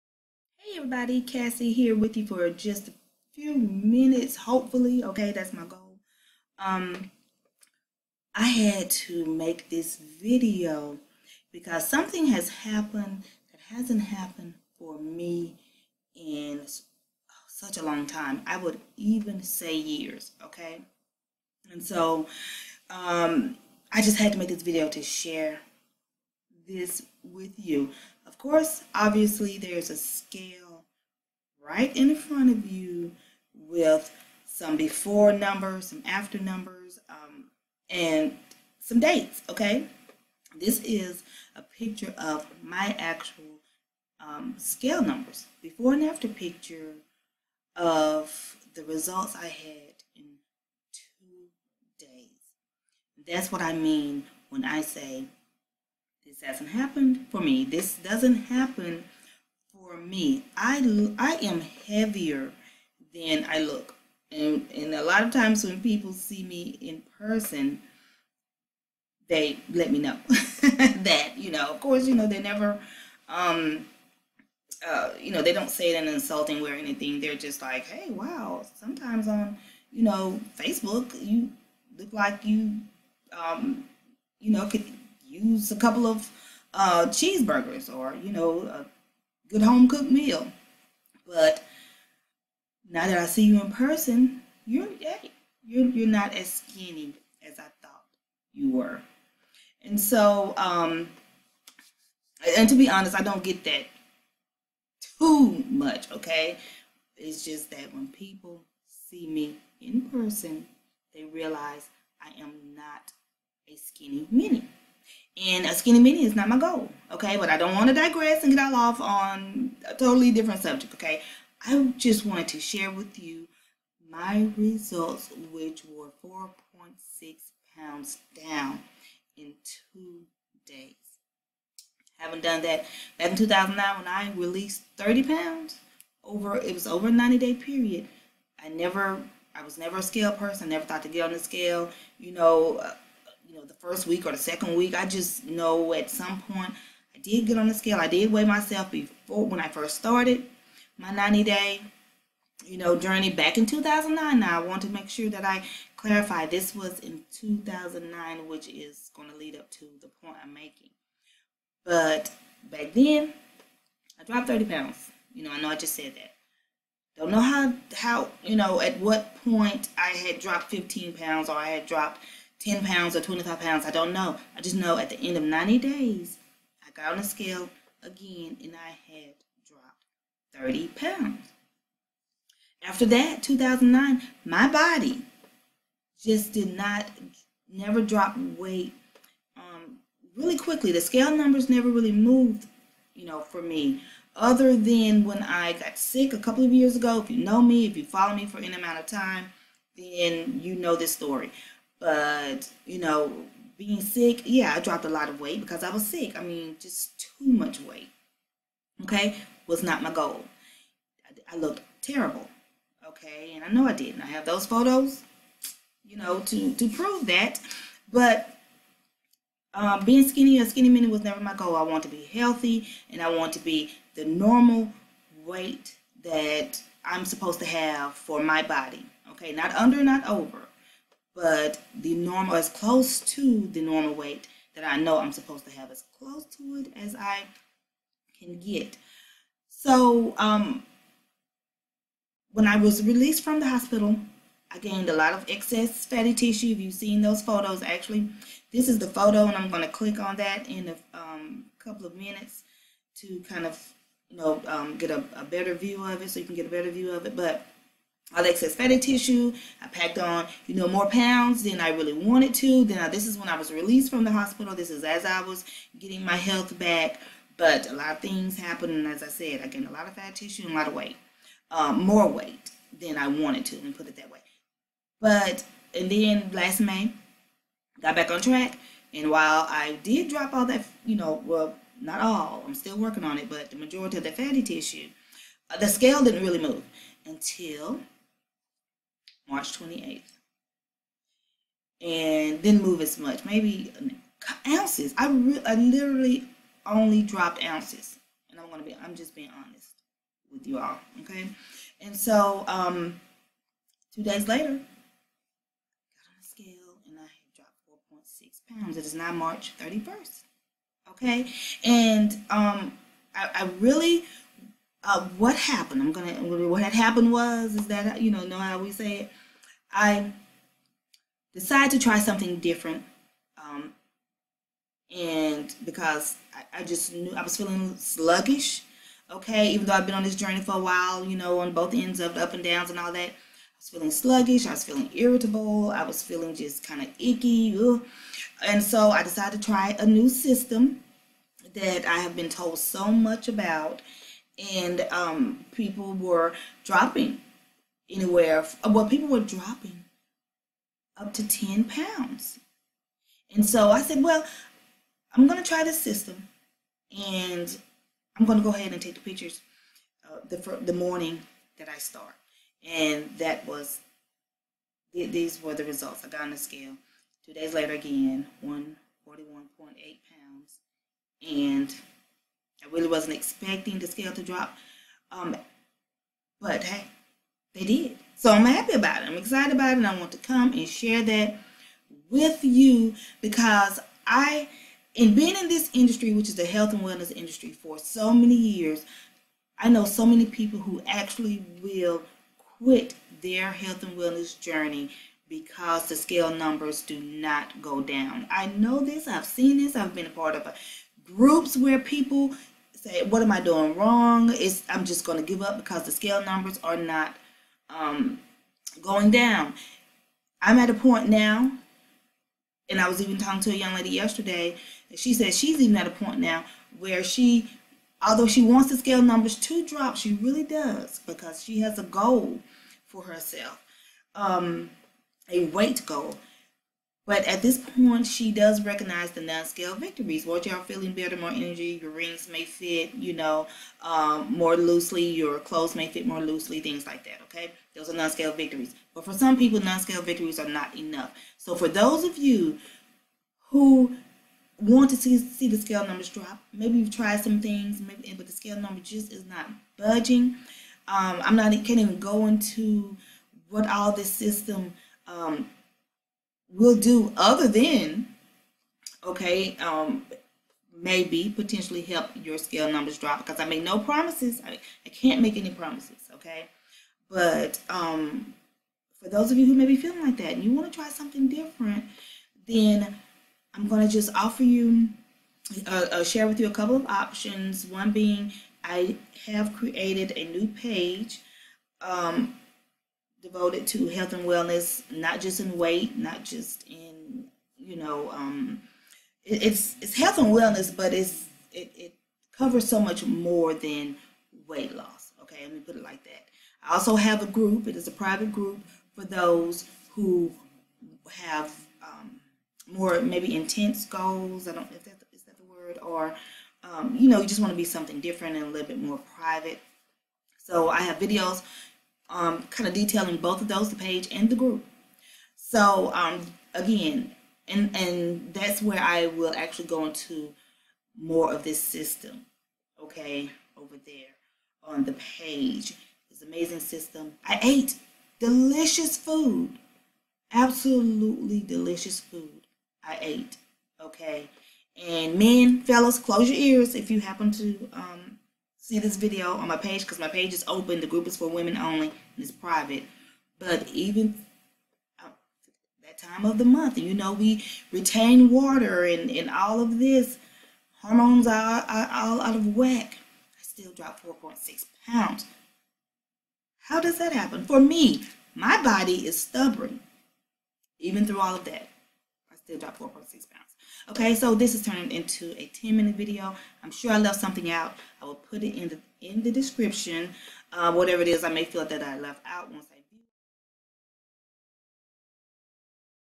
Weight Loss Plateau: Everybody Is Different
Weight Loss Plateaus happen to even the most committed of us.
The scale is not my friend but we became better associates yesterday!😉

Like. Follow. Contact.